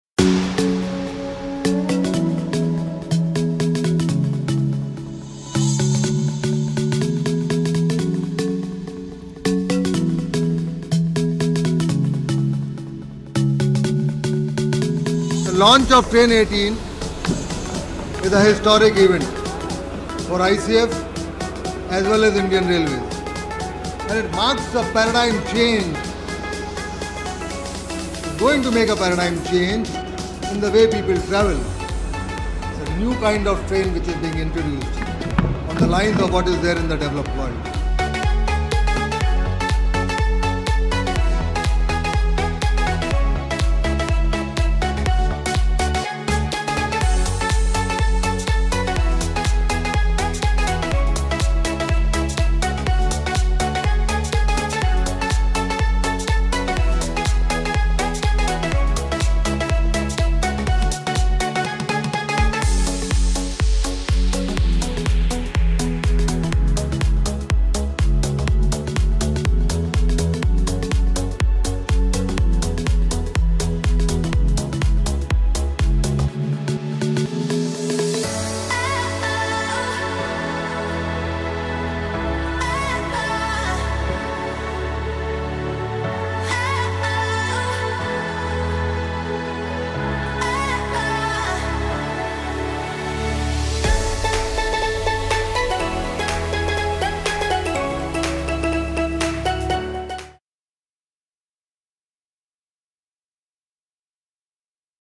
Train 18, the NextGen Shatabdi, makes debut
Train 18, the much anticipated Indian-made trainset was unveiled to the public by Indian Railway Board chairman Ashwani Lohani.

The gleaming, blue-nosed train has a cone-shaped frontage, similar to a bullet train. The new train comes fitted with amenities on a par with the best in the world — from on-board Wi-Fi to GPS-based passenger information system, ‘touch-free’ bio-vacuum toilets, LED lighting, mobile charging points, and a climate control system that would adjust the temperature according to occupancy and the weather.